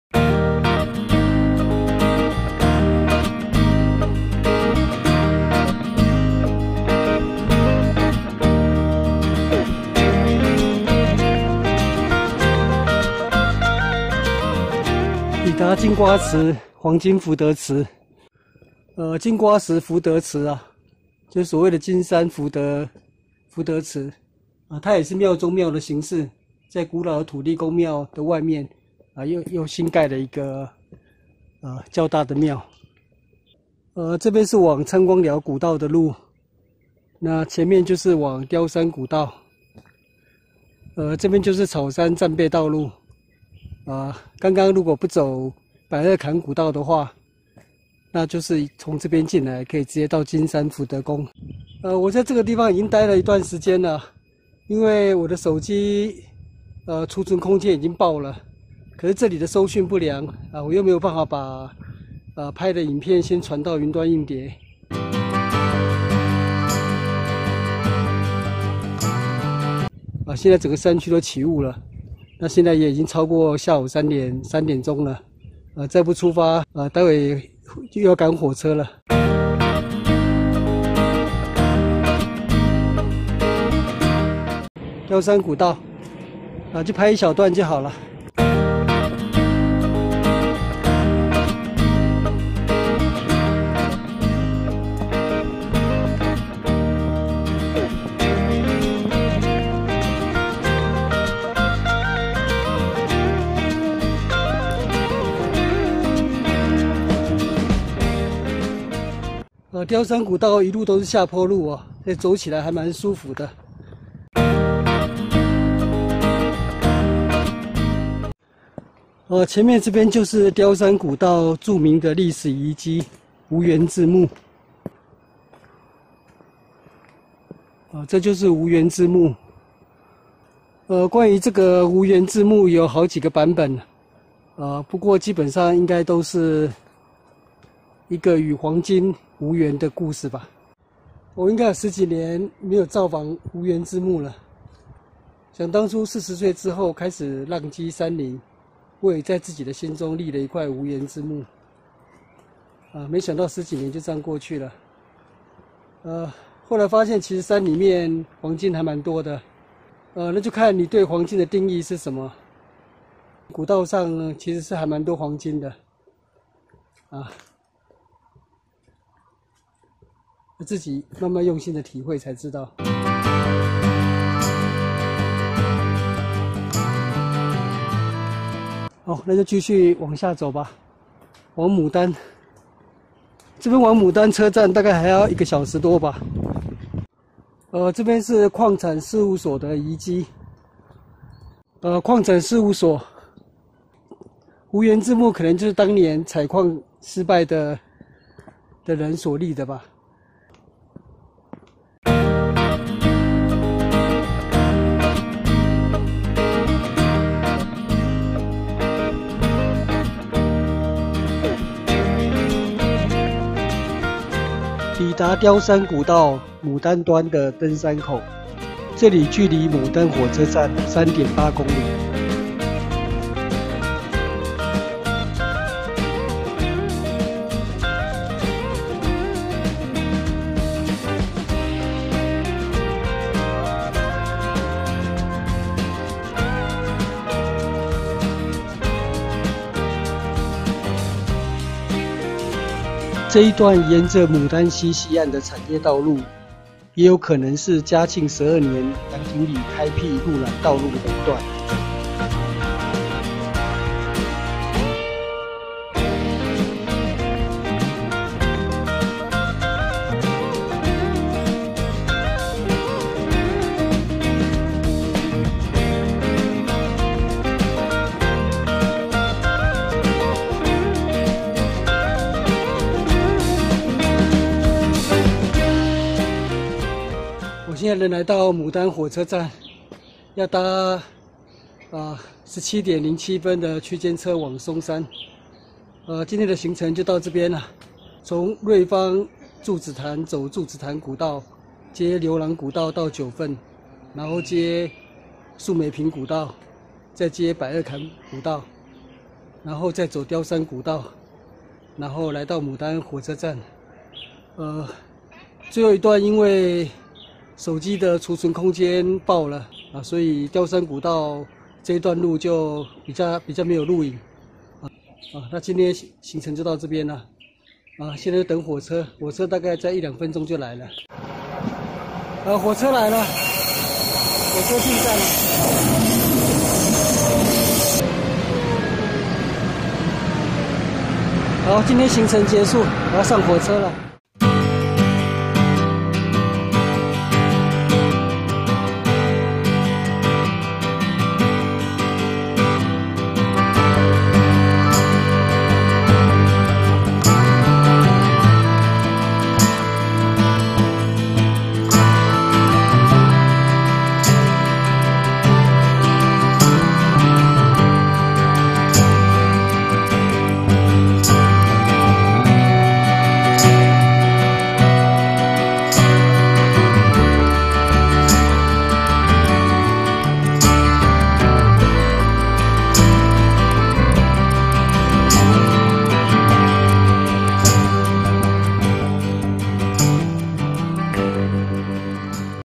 牡丹坑溪採礦舊路，充滿神祕傳奇的無緣之墓～貂山古道 ★淡蘭古道06
貂山古道緣於日治時代明治三十一年（1898年）因牡丹溪支流發現砂金，繼而發現金礦，先後築造運送礦產之輕便車路，興建礦務所、醫療站等設施。這條山路也成金瓜石、九份與雙溪、牡丹之間往來必經之路。古道的途中的無緣之墓是主要的歷史遺跡。
貂山古道的交通資訊，請參考以下Google Map連結：
（Please refer to the following Google Map link for the traffic informatin ）
【相關影片】
更詳盡的旅記內容，請參考我的網站：
Tony的自然人文旅記